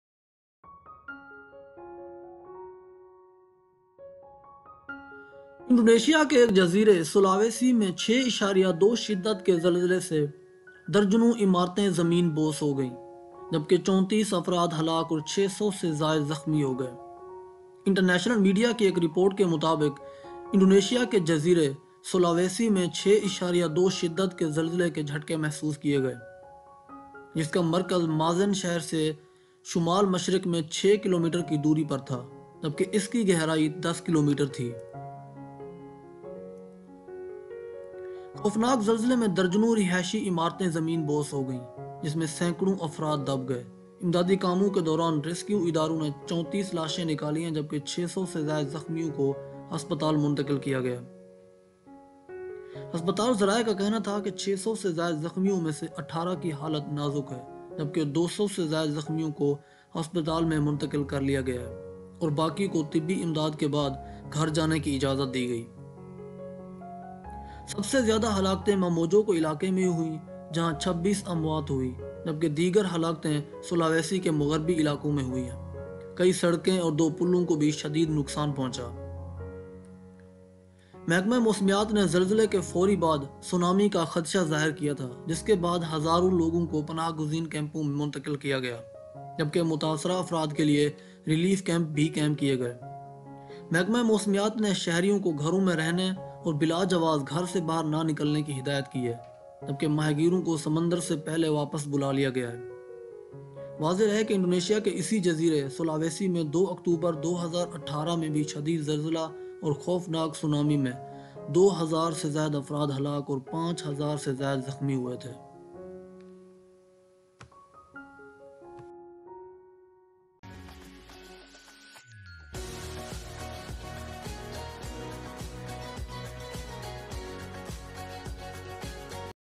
Indonesia Earthquake 2021 | Indonesia Earthquake and sunami 2021
Indonesia Earthquake 2021:   Indonesia earthquake kills at least 35, injures hundreds, A 6.2-magnitude earthquake on Indonesia’s Sulawesi island killed at least 35 people and injured hundreds on Friday, with the meteorological agency warning of aftershocks, possibly strong enough to trigger a tsunami.

The powerful earthquake in Indonesia struck six kilometers northeast of the town of Majene, at the relatively shallow depth of 10km, just before 1:30am (local time), sending thousands of frightened residents out of their homes and fleeing for higher ground.